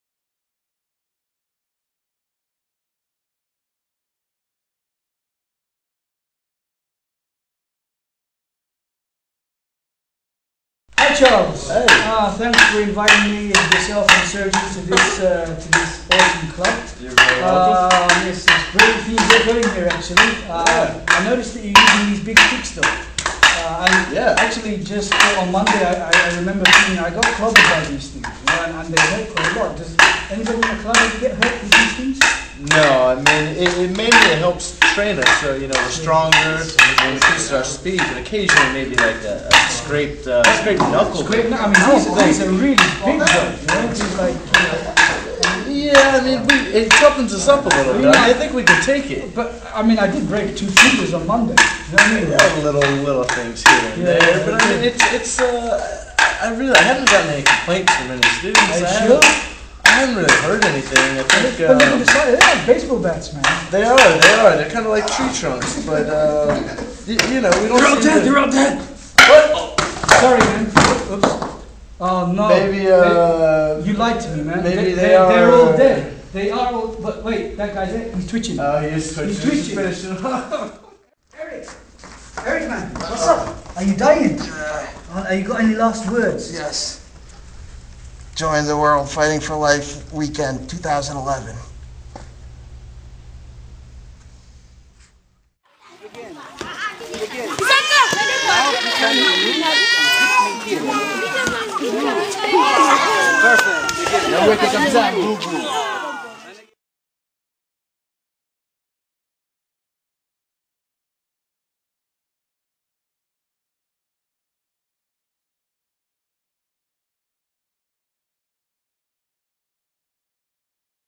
Orlando Eskrima Advert For WORLD FIGHTING FOR LIVES WEEKEND 2011
- JOIN THE 2nd WORLD FIGHTING FOR LIVES WEEKEND (12th-13th November 2011)... HELD SIMULTANEOUSLY AROUND THE WORLD to raise money for our projects for street children and orphans in Kenya, Sierra Leone and Malaysia (more in Ghana and Bangladesh in the plans). we need to raise money for a stable home in Kenya for 50 children... help us... get your club involved in the WORLD'S MOST GLOBAL MARTIAL ARTS EVENT

Many thanks to my brothers and sisters at Orlando Eskrima, FL, USA for their hospitality, support and the good times we shared :)